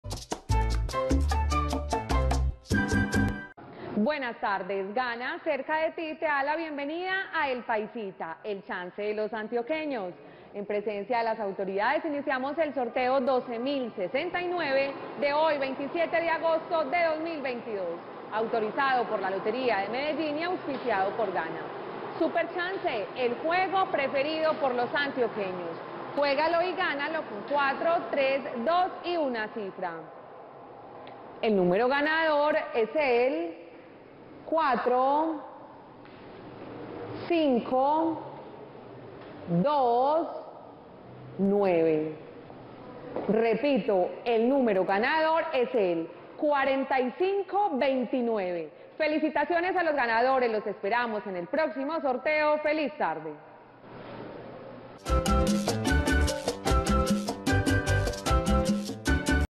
PAISITA DÍA Resultados del SÁBADO 27 DE AGOSTO de 2022 ✅🥇🔥💰
Hola buenas tardes, te presentamos los resultados de las loterías y chances en Colombia, vamos con el Resultados del Chance Paisita día del SÁBADO 27 DE AGOSTO de 2022.